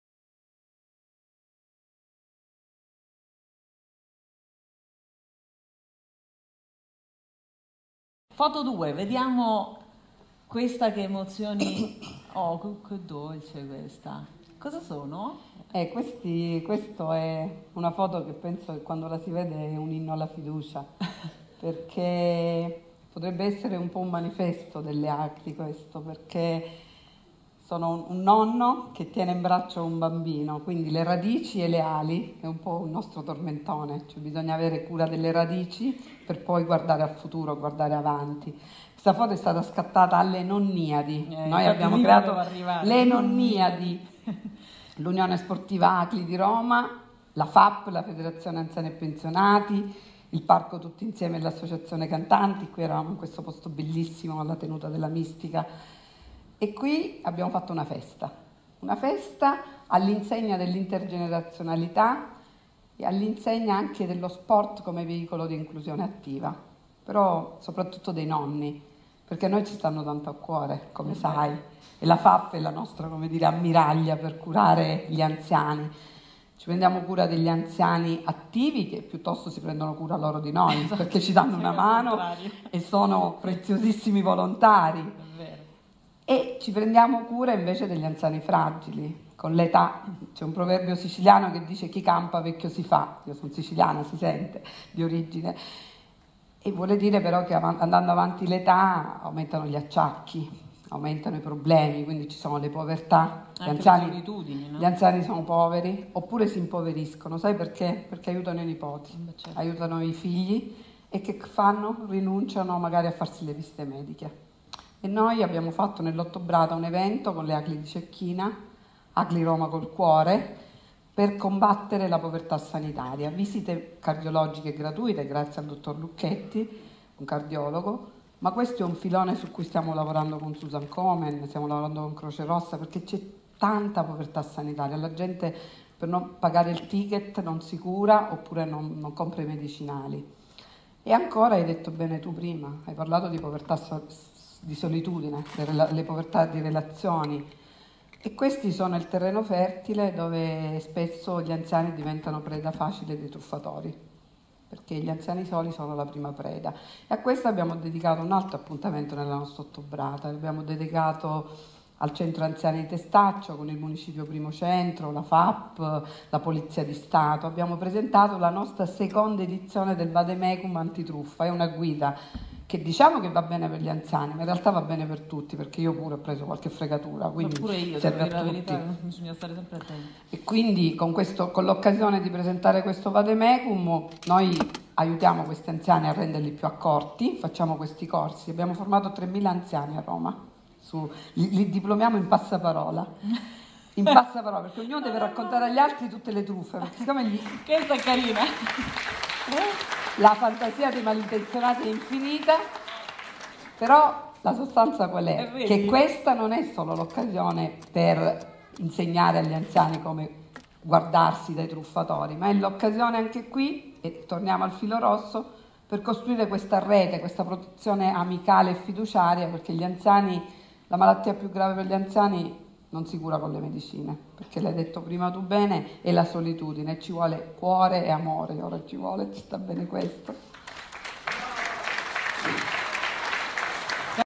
IV Ottobrata Solidale - Video intervista a Lidia Borzì - Foto 2 "I legami intergenerazionali"
Evento finale della IV Ottobrata solidale, l'intervento della presidente delle ACLI di Roma, Lidia Borzì si è articolato attraverso una foto intervista in sei parti.